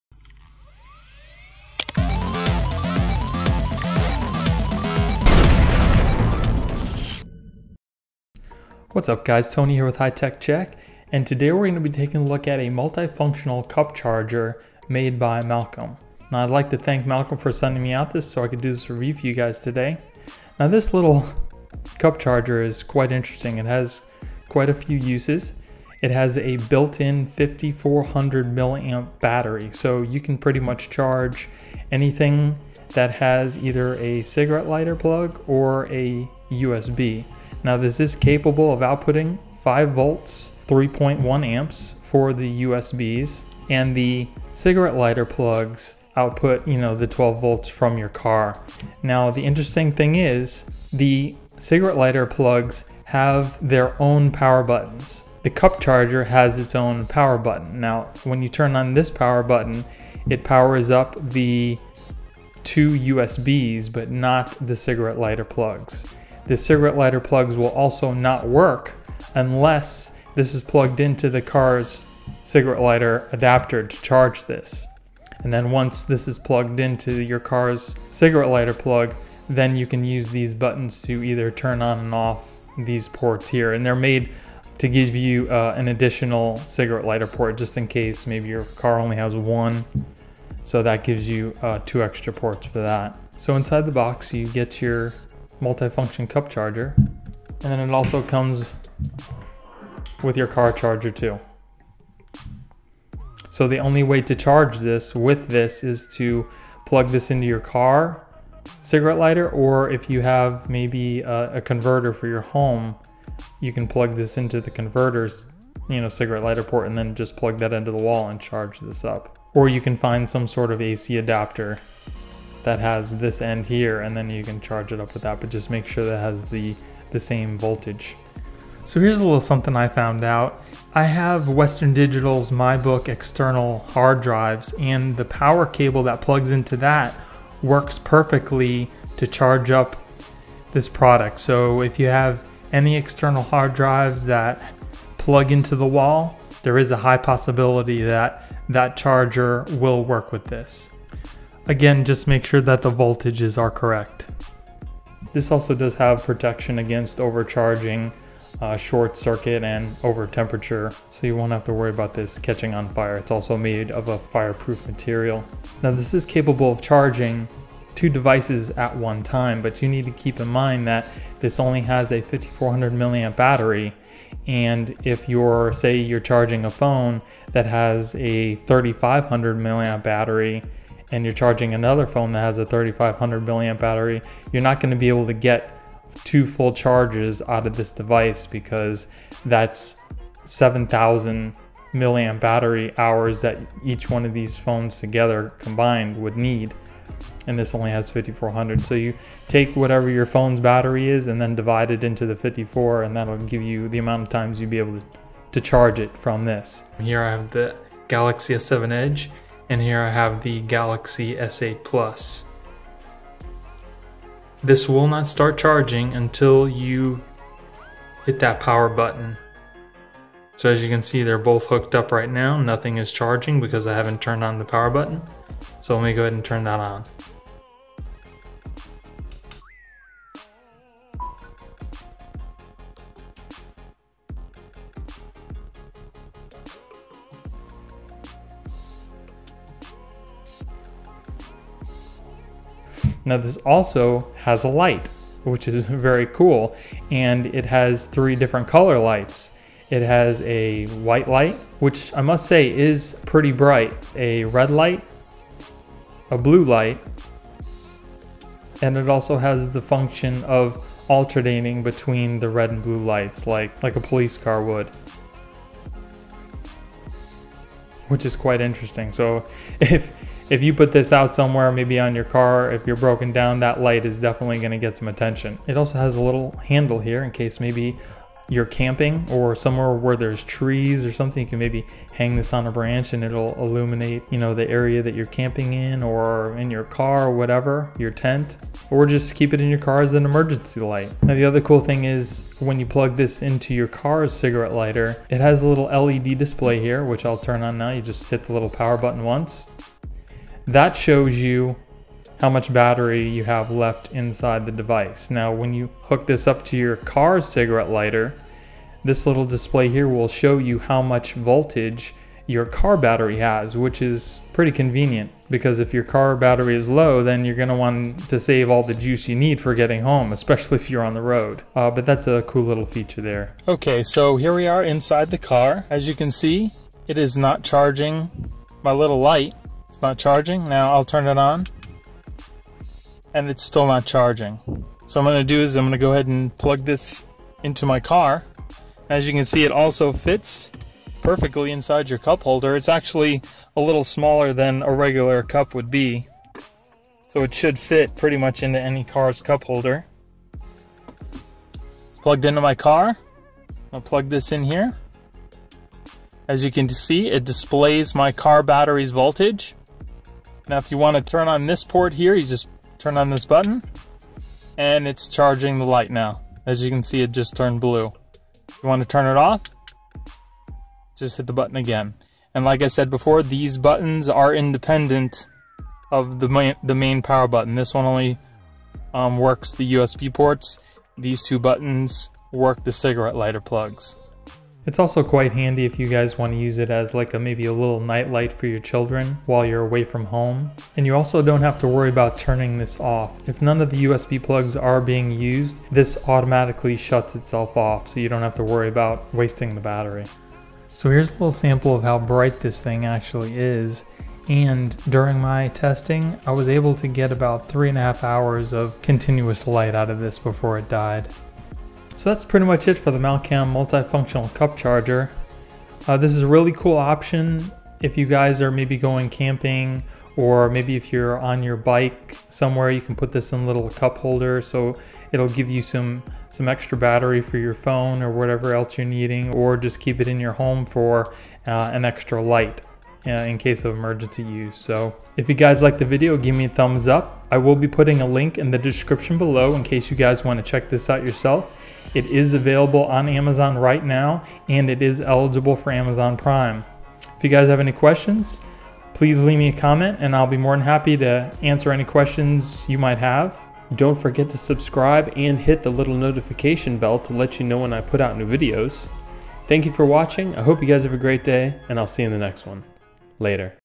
Car Cup Charger By Malcam - Demo & Review
Malcam 12-24V 60W Multi-Functional Cup Car Charger with 2 Cigarette Lighter Sockets,3.1A Dual USB Charging Ports,5400mAh Power Bank and Warning/Flashing Light

Our Advantage: We have a professional research and development team for the car charger.Provide you 2 cigarette lighter sockets with separate switch and 3.1A dual USB charging ports.Allow you to charge multiple devices simultaneously.Great for your kindles,iPhones, iPads, tablets, MP3 players, cameras, and other mobile devices.
Built-in Power Bank&LCD Display: There's 5400mAh polymer battery inside the product.You can charge your phone 2~3 times fully.LCD display shows the car battery voltage(when it's connected to the car) and remaining power bank battery.
Practical Design: There are multiple modes of your choice:1.The white/blue/red light is always on. 2.The red and blue light alternately flash.It can be used as warning/flashing Light in many cases.Removable cable and handle design make the product easy to carry.
Full Multiple Protections:Intelligent protection system for overcharging,over-current,short-circuit,short-circuit,over-temperature,over-voltage,over-power.Provide further protection for your use
Excellent Quality: Product body is made of fire-proof material.The connecting cable is also fire-proof and certified by UL.